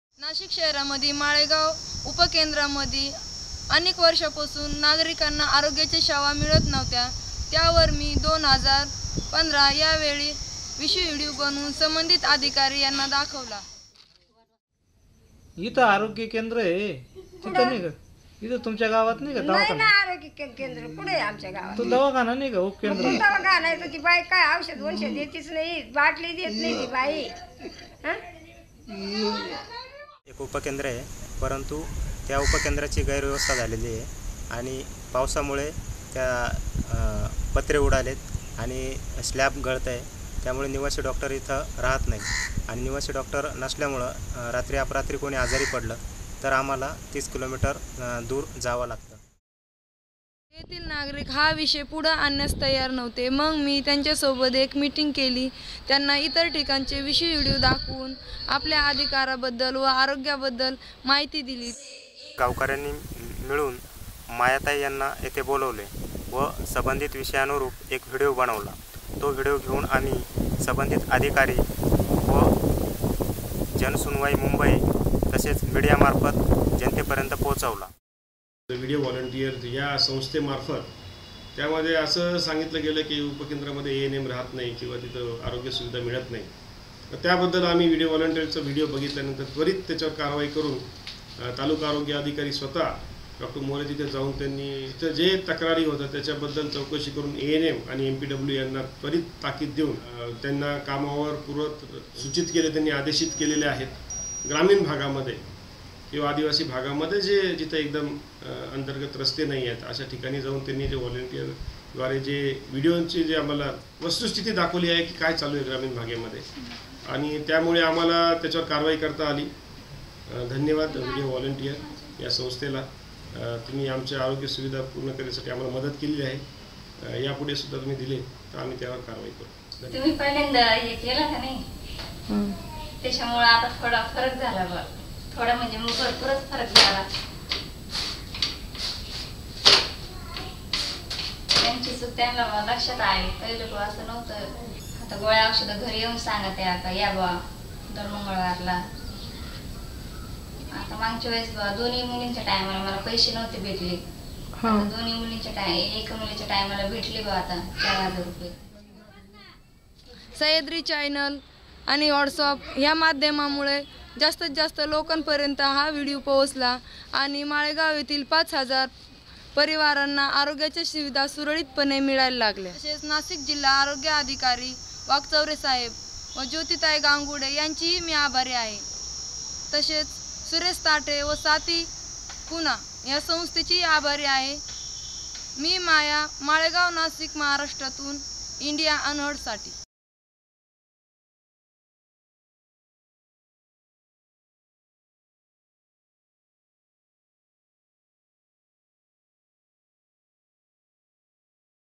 Thats How Camera Revives the PHC in Malegoan, Maharashtra - Maya Khodve reports for IndiaUnheard
Community correspondent Maya khodve reports from Maharashtra for Video Volunteers.